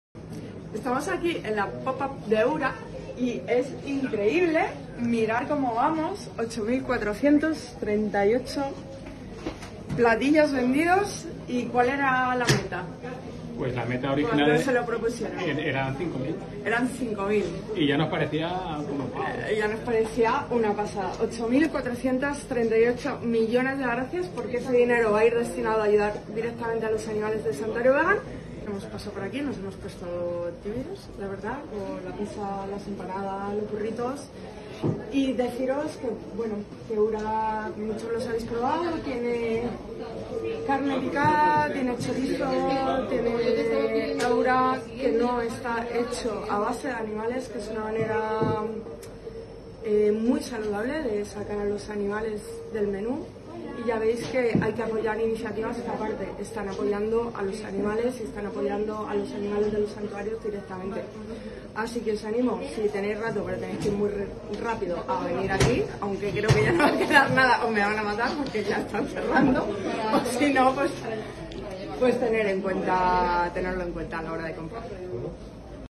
Gracias a Heura por este increíble evento solidario con Fundación Santuario Vegan
La pop up de Heura Foods ha sido todo un éxito!
De 5000 platillos que se puso de meta Heura habéis alcanzado los 9436 platos vendidos! Lo que significa 9436 € donados a la Fundación Santuario Vegan.

Gracias a todo el equipo de Heura que no han perdido la sonrisa a pesar de la sobrecarga de trabajo ni la motivación para ayudar a los animales.
Y sobre todo gracias por pasaros por allí.
Pocas empresas tienen iniciativas para ayudar directamente a los animales considerados de granja, por eso tenemos que volcarnos.
¡Gracias Heura!!!

Por favor, ayúdanos a salvar vidas 🙏💚